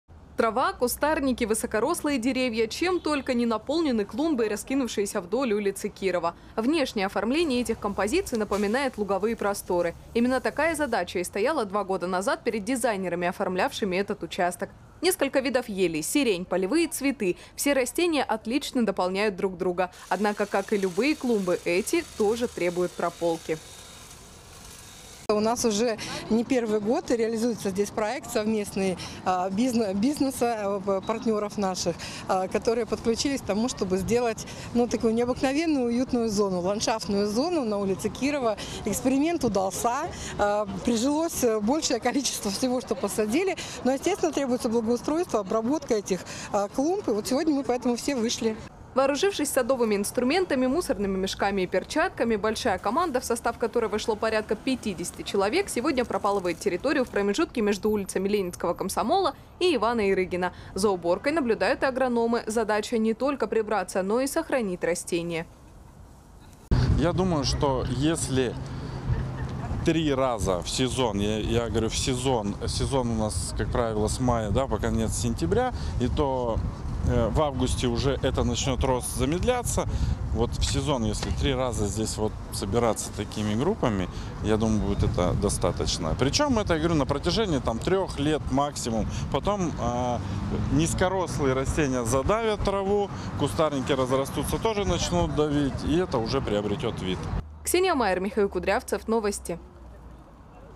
Клумбы на улице Кирова, где высажены больше 2 тысяч растений, привели в порядок - Абакан 24
Большой субботник на улице Кирова в Абакане. Администрация города, совместно с подведомственными учреждениями, навели порядок на клумбах — на участке от Ленинского Комсомола до Ивана Ярыгина. На всей протяженности Кирова высажено более 2 тысяч растений  Пропалывать траву и убирать сорняки здесь нужно не чаще трех раз за сезон, - говорят специалисты.